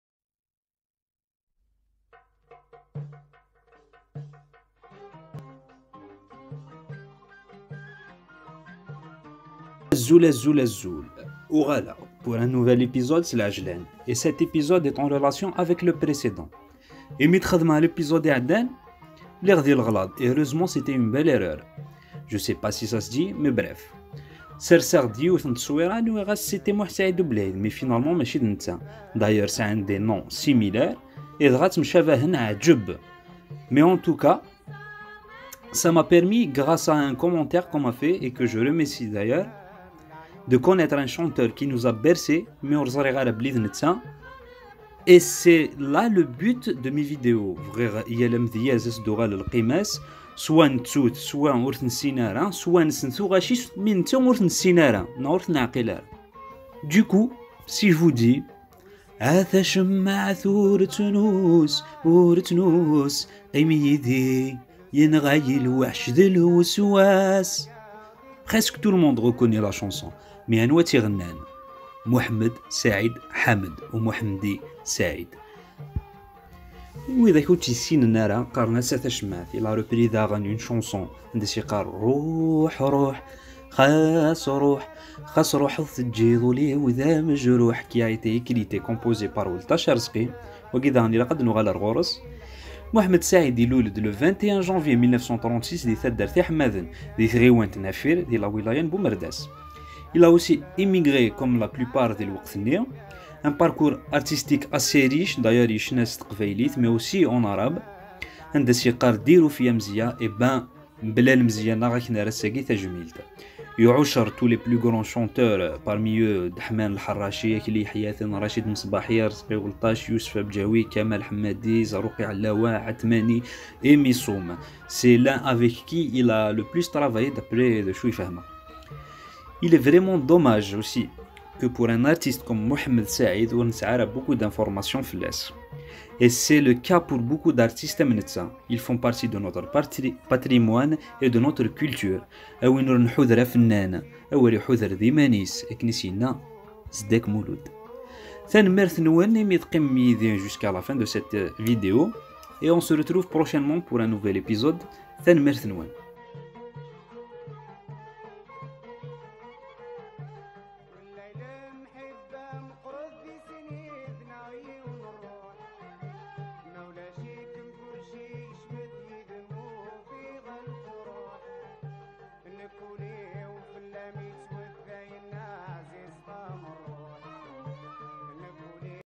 Mohamed said (chronique, histoire) episode 2
Un chanteur kabyle qui nous a bercer avec ses multiples chanson. petite histoire en video